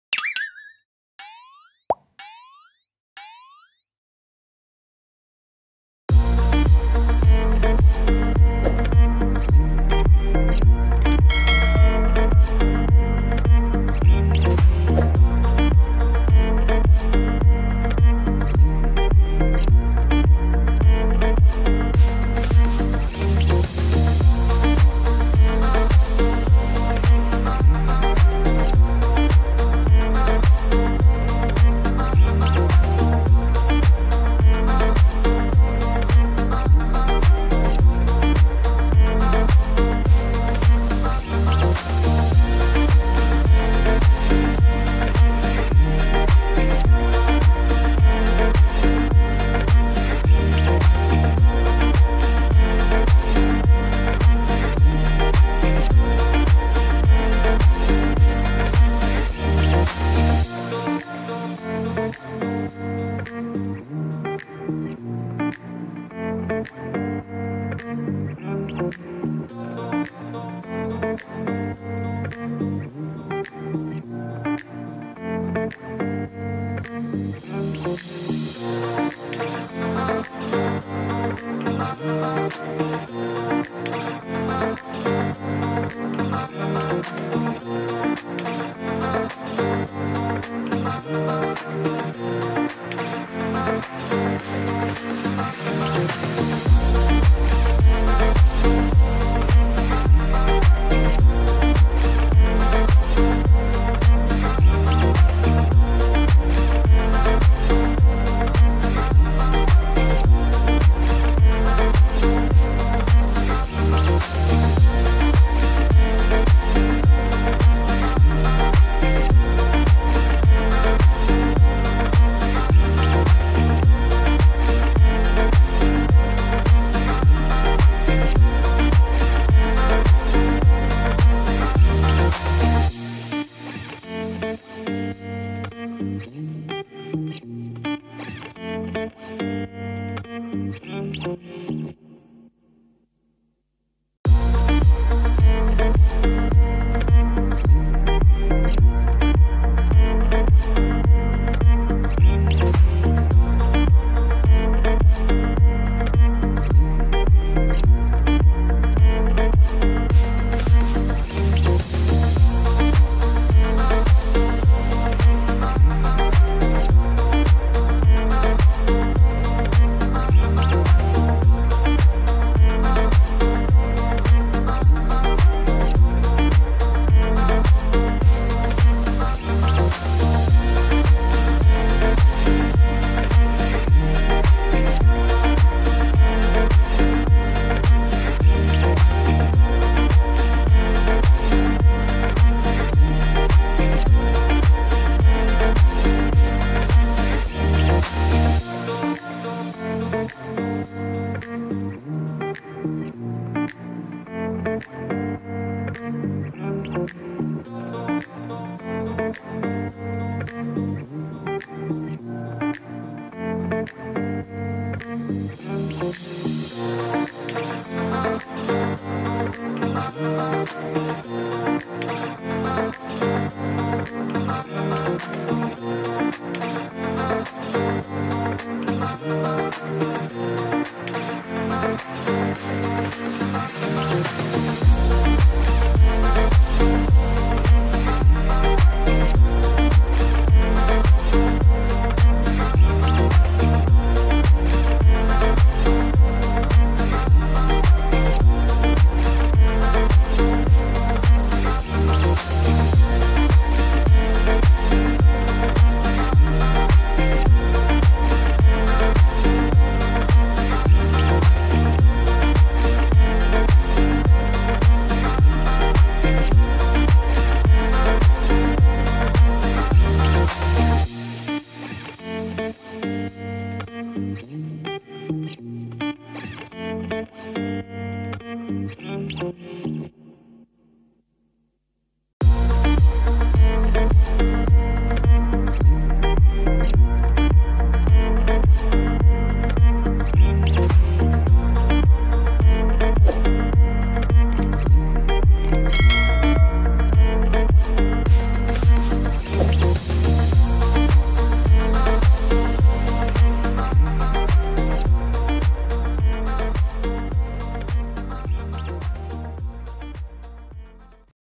melukis Ochobot guna pen marker - Boboiboy Galaxy
⭕The smartphone I use:
LG G7 ThinQ
⭕What app are you using:
Kinemaster Pro/PicArt(edit picture and thumbnail)

✨I using..
-Paper(A4 120gsm )✅
-Pencils ✅
-Pen Artline(Unicorn) ✅
-Pen Maker Art(Touch Cool) ✅
✨ MUSIC🎶